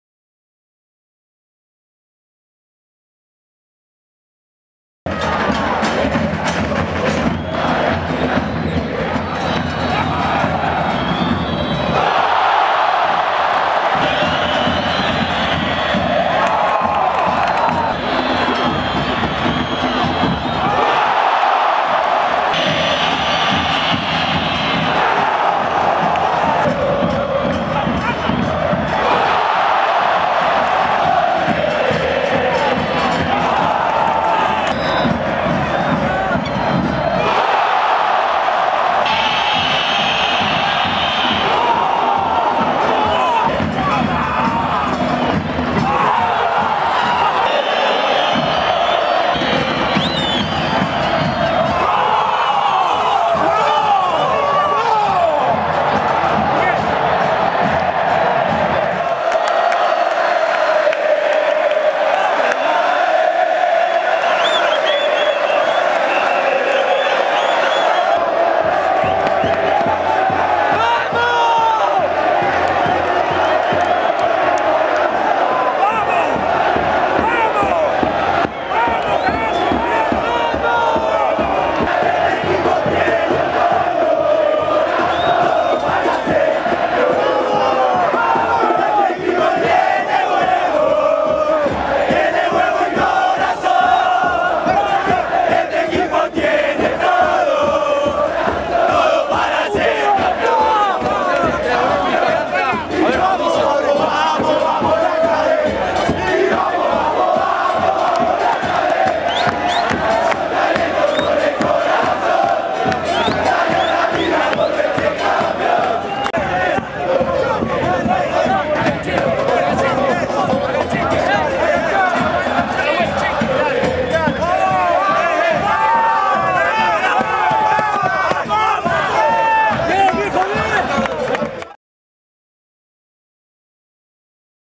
Penales y festejos de River - Rosario Central
El Canalla ganó por 5-4 en la definición, luego de un 0-0 en los 90 reglamentarios, en el estadio San Juan del Bicentenario, por los Cuartos de la Copa Argentina Sancor Seguros.